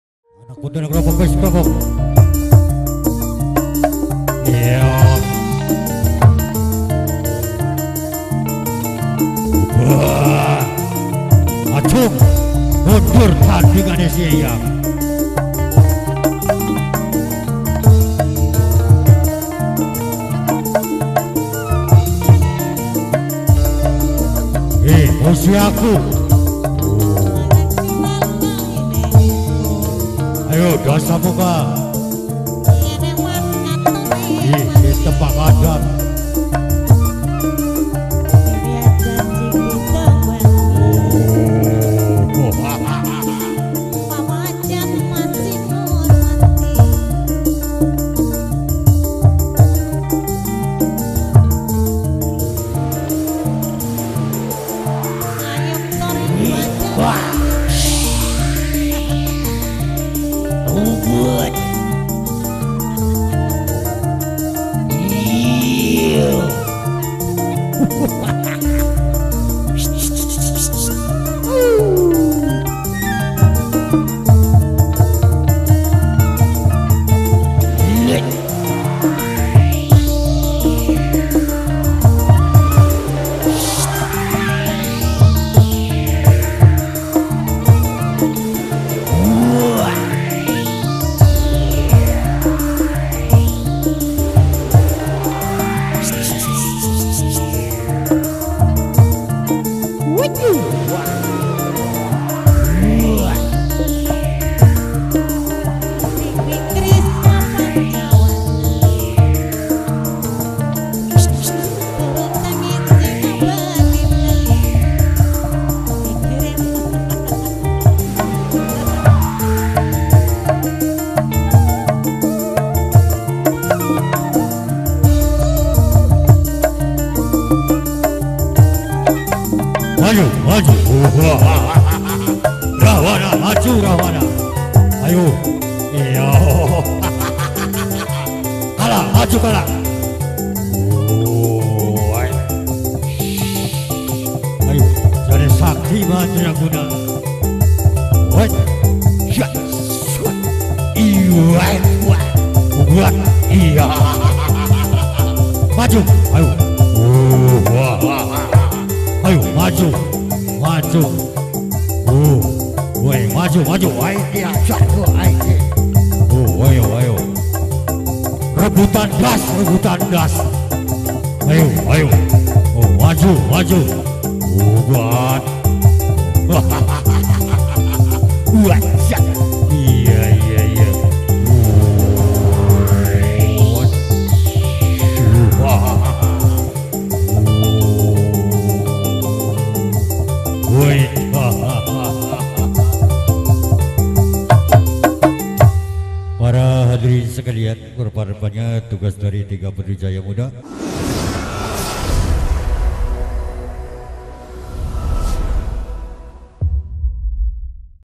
Rahwana VS Cicimilitan | Burok TPJ Live Ds. Pengarengan
Khitanan - Ulul Ajmi
Keluarga - Bpk.Tarmudi / Ibu Wijawanti
Lokasi Hajat - Ds.Pengarengan Kec.Pangenan Kab.Cirebon
Date - 03 Agustus 2019

Alamat - Burok TPJ, D/a.Kalibangka Lor Kec.Pangenan Kab.Cirebon
Pimp.Bpk.Raid